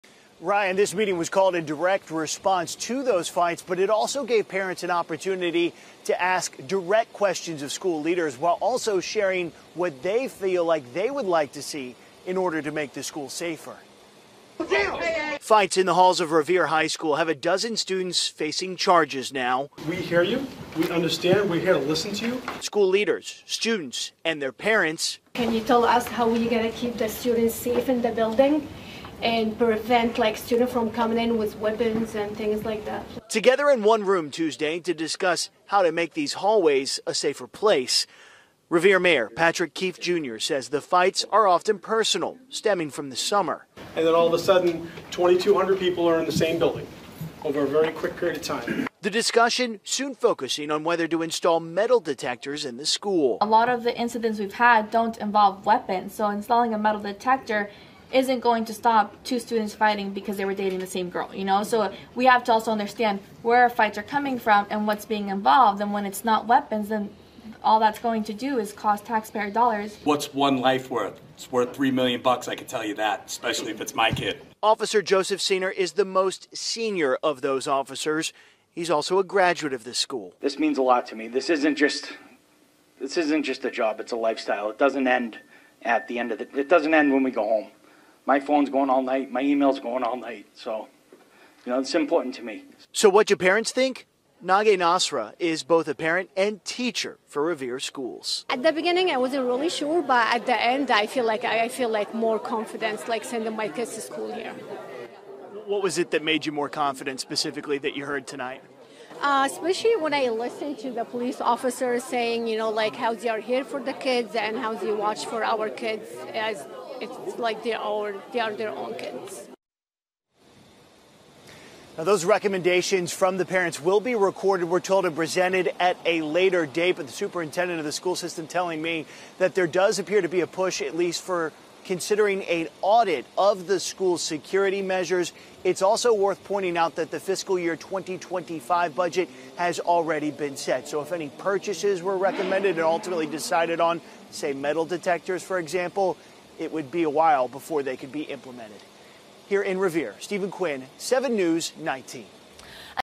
Parents, students, staff meet to discuss fights at Revere High School
Parents, students, staff meet to discuss safety protocols in wake of recent fights at Revere High School